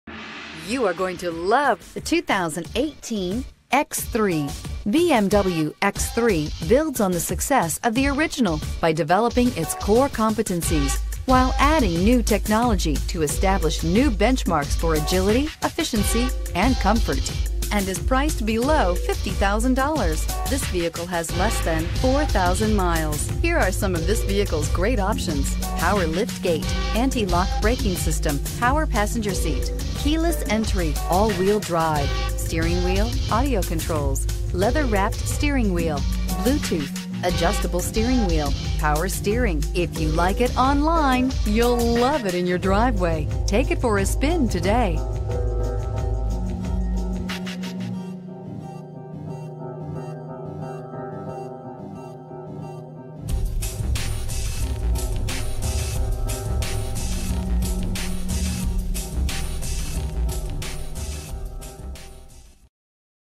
2018 BMW X3 15810L
2018 BMW X3 Wayne, NJ
Stock# 15810L

BACK-UP CAMERA, ALL WHEEL DRIVE, BLUETOOTH, MP3 Player, NO ACCIDENTS on CARFAX, KEYLESS ENTRY, 29 MPG Highway, TURBOCHARGED, POWER LIFTGATE, ALLOY WHEELS.